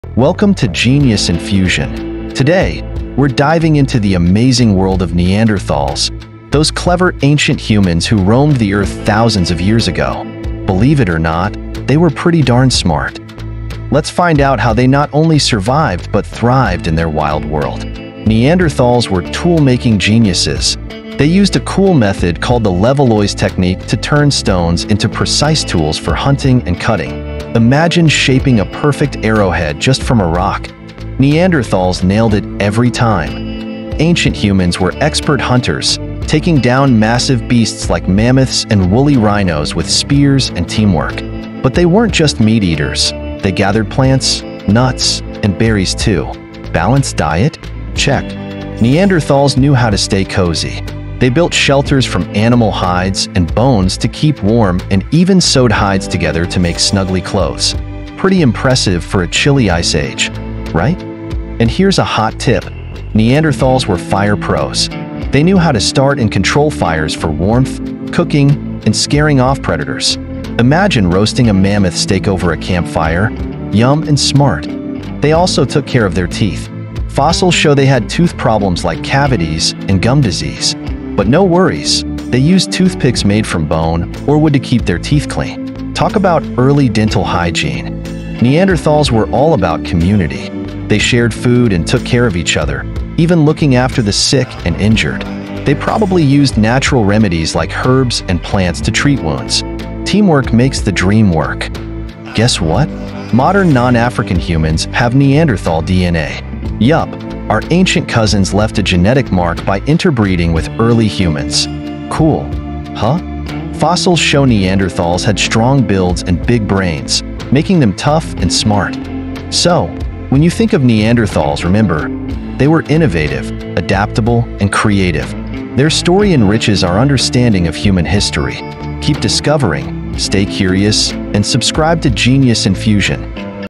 Secrets Of The Neanderthals
Neanderthals: Clever Innovators | Adaptable Survivors | Creative Thinkers!

In this video, we’re diving deep into the world of Neanderthals, those fascinating ancient humans who lived thousands of years ago. You’ll be amazed by how much more there is to their story than just living in caves! Neanderthals were incredibly intelligent, resourceful, and creative, and their legacy continues to impact us today. Get ready to uncover the secrets of their tools, hunting strategies, use of fire, and even their artistic expressions.

Discover the Genius of Neanderthals

🪓 Tool-Making Masters: Neanderthals were true pioneers in tool-making. Using the sophisticated Levallois technique, they could craft tools from stones with remarkable precision. This method involved preparing a stone core to produce sharp, uniform flakes. Such tools were crucial for hunting and everyday tasks. Imagine shaping a perfect spearhead or scraper from a simple rock—that's what Neanderthals achieved with their advanced skills. Their ingenuity in tool creation reveals a deep understanding of materials and an ability to innovate.

🍖 Expert Hunters & Gatherers: These ancient humans were also exceptional hunters. They employed a variety of strategies to hunt large game, such as mammoths and woolly rhinos. Fossil evidence shows they worked in groups, using their tools to bring down these massive animals efficiently. But Neanderthals didn't just rely on hunting. They were adept gatherers as well, foraging for plants, nuts, and berries. This balanced diet helped them survive and thrive in diverse environments, showcasing their adaptability and understanding of the natural world.

🏕️ Shelter & Clothing Innovators: Neanderthals knew how to stay warm and protected. They built sturdy shelters using materials like animal hides, bones, and wood. These shelters provided much-needed protection from harsh weather conditions, especially during the Ice Age. Moreover, Neanderthals likely created clothing by sewing together hides using bone needles. This ingenuity in crafting clothing highlights their ability to adapt to their surroundings and maintain body heat in freezing temperatures.

🔥 Masters of Fire: Fire was a crucial element in Neanderthal life. They had mastered the art of starting and controlling fires, which served multiple purposes: providing warmth, cooking food, and even warding off predators. Fire allowed Neanderthals to cook meat, making it easier to digest and providing more energy. This control over fire was not just a survival skill but a transformative technology that enhanced their daily lives. Picture a Neanderthal family gathered around a fire, sharing a meal and staying warm—that’s how central fire was to their existence.

🦷 Early Dental Care: Believe it or not, Neanderthals practiced dental hygiene! Fossil evidence shows that they suffered from dental issues like cavities and gum disease. However, they didn’t just endure these problems—they actively tried to manage them. Evidence suggests that Neanderthals used toothpicks made from bone or wood to clean their teeth. This early form of dental care indicates their awareness of health and self-care practices. They were ahead of their time in understanding the importance of maintaining oral hygiene.

🤝 Community & Compassion: Neanderthals lived in tight-knit groups, caring for their sick and injured. Fossils show they likely used natural remedies like herbs for treatment, highlighting their strong social bonds and teamwork.

🎨 Artists at Heart: Surprisingly, Neanderthals were creative! They painted caves and made jewelry from shells, showing their appreciation for beauty and symbolism.

🔬A Lasting Legacy: Modern non-African humans carry Neanderthal DNA. Their interbreeding with early humans has left a genetic legacy that influences us even today.

In this video, we delve into these aspects of Neanderthal life, exploring their ingenuity, resilience, and contributions to human evolution. From tool-making to community care, Neanderthals exemplified traits that are fundamental to humanity.